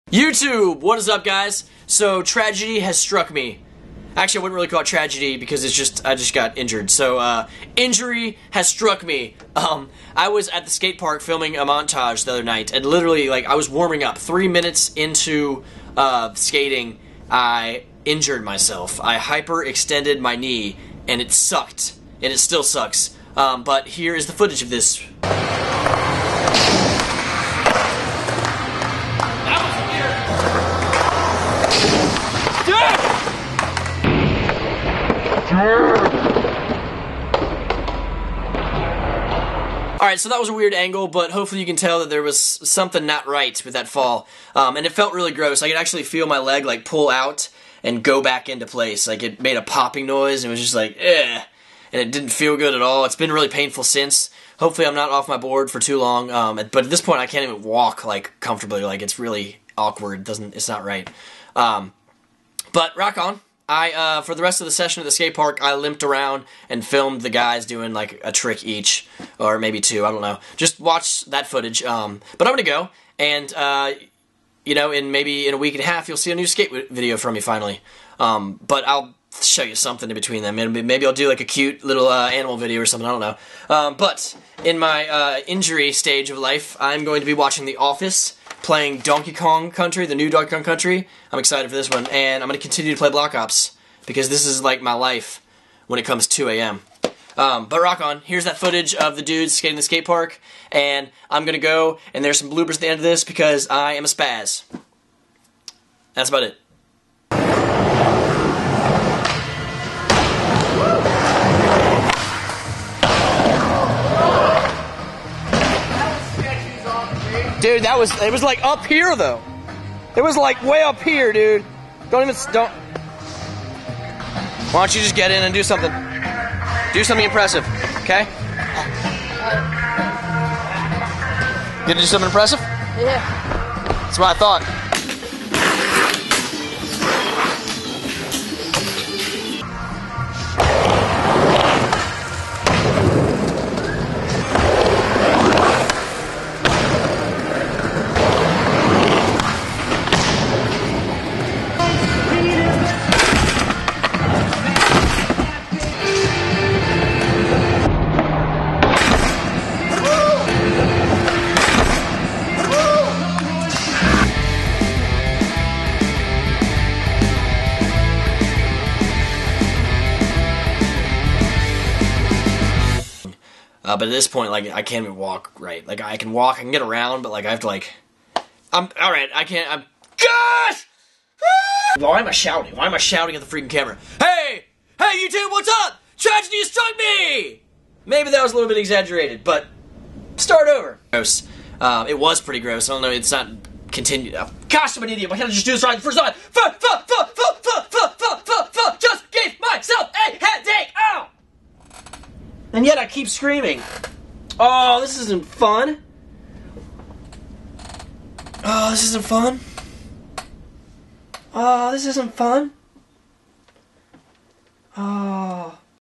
I Hyper-Extended My Knee!
- My board company's site!

I filmed this video a few days ago and my knee has actually gotten much better. So all is well and I'll be back on my board soon! Hope the winter is treating everyone alright!